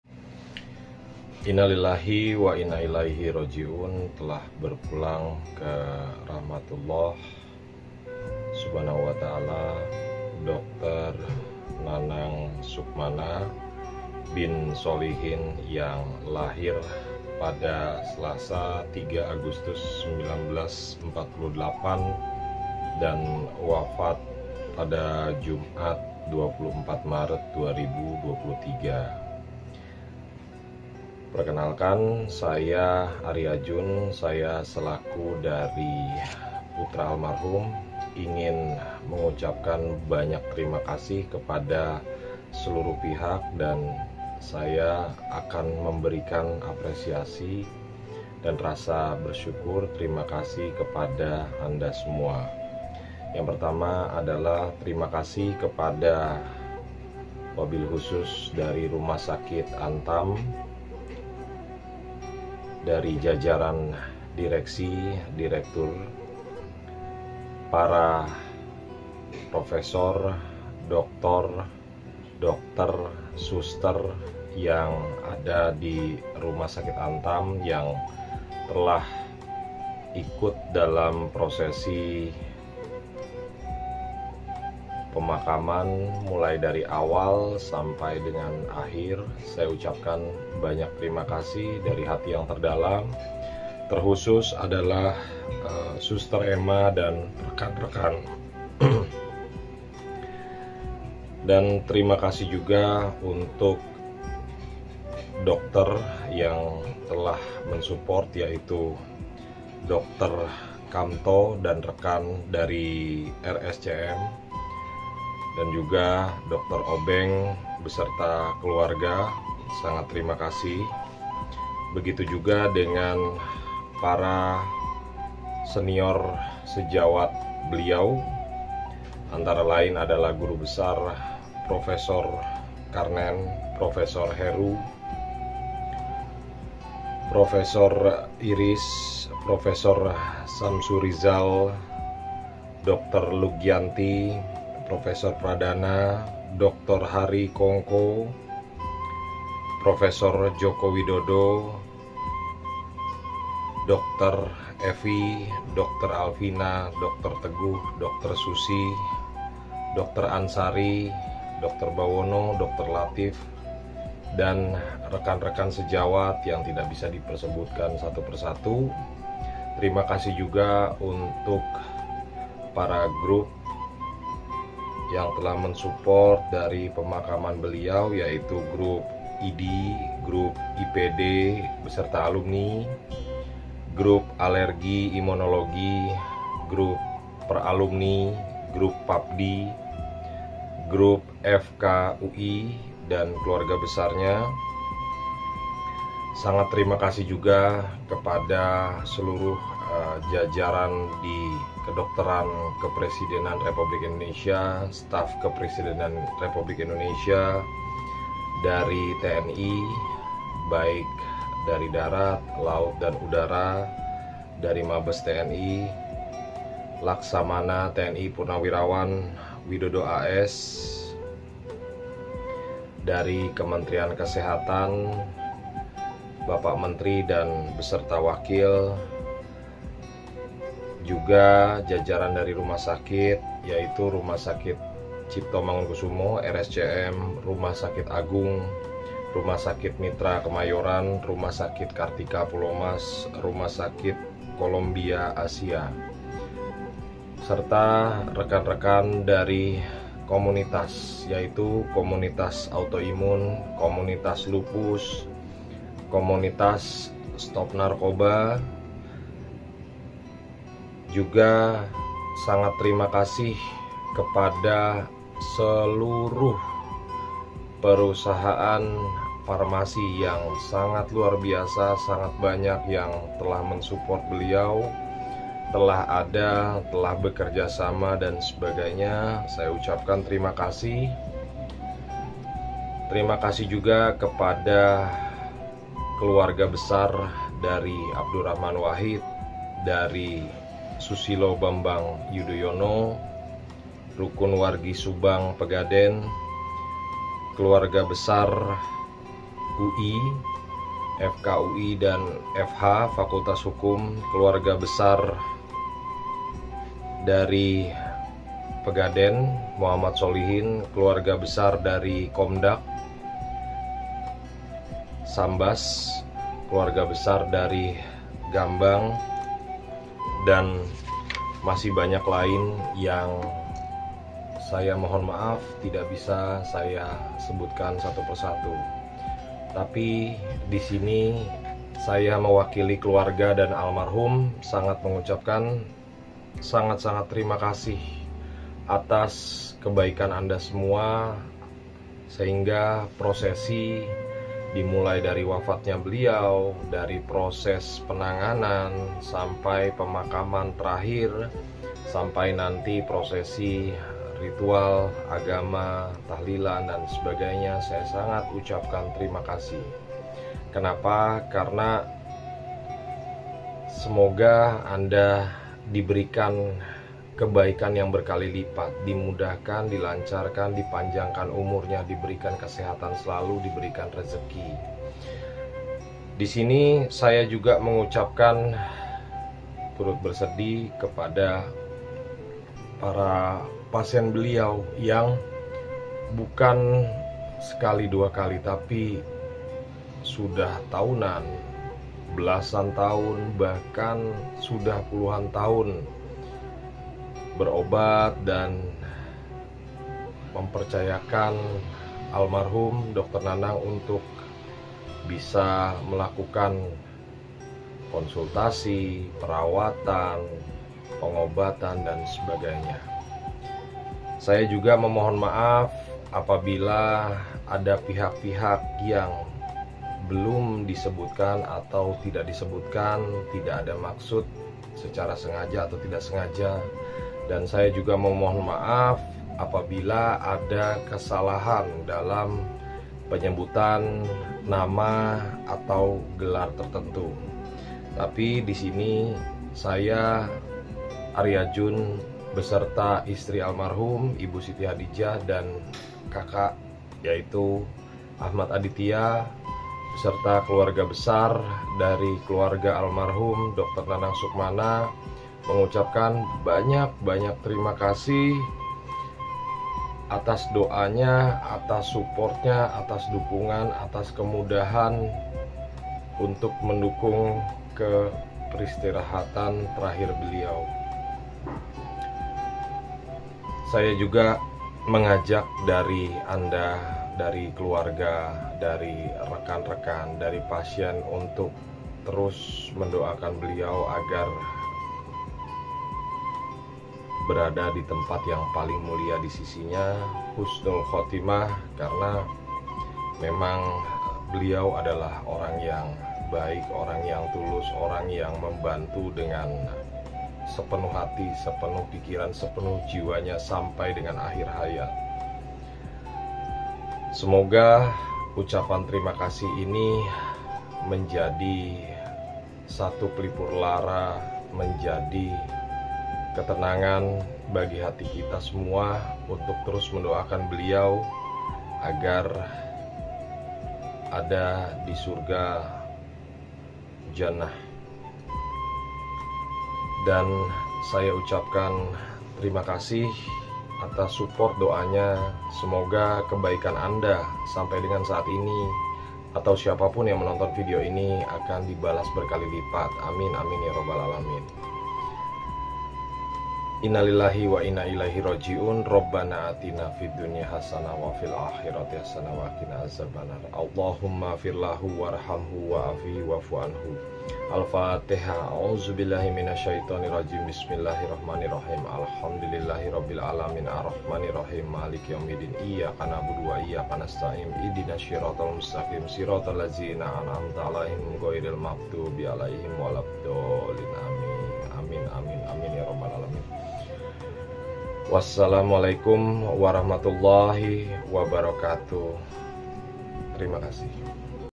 DOKTER SPESIALIS PENYAKIT DALAM TERBAIK DR. NANANG SUKMANA Sp.PD. K-AI. FINASIM | TERIMA KASIH
DOKTER SPESIALIS TERBAIK DR. NANANG SUKMANA Sp.PD. K-AI. FINASIM

TERIMA KASIH DARI KELUARGA ALMARHUM DR. NANANG SUKMANA

Atas semua dukungan, support, bantuan, doa dan sebagainya, dari mulai awal sampi ke tempat peristirahatan terkhir beliau

Semoga Amal Ibadah Anda dibalas bekali lipat oleh Alloh SWT.

Mohon teriring doa selalu dan mengirimkan AL - FATIHAH.

AMIN